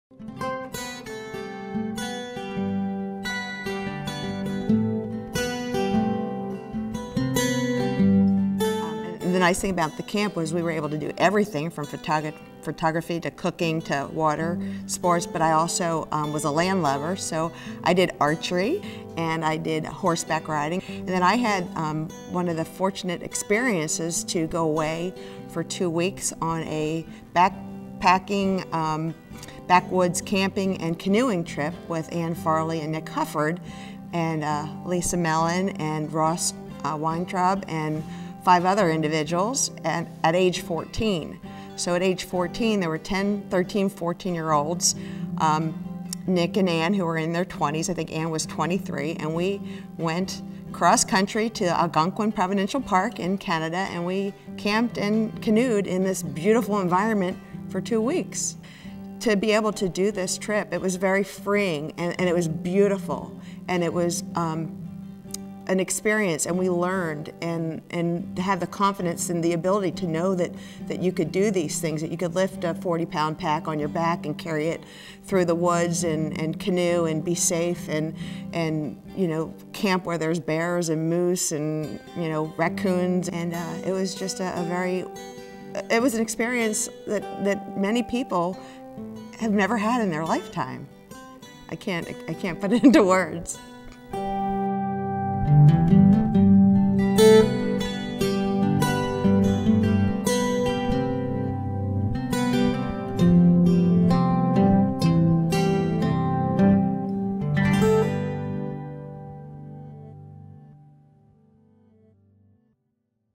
Adventure
Lynn Slovin shares some of her adventures while at Camp Tockwogh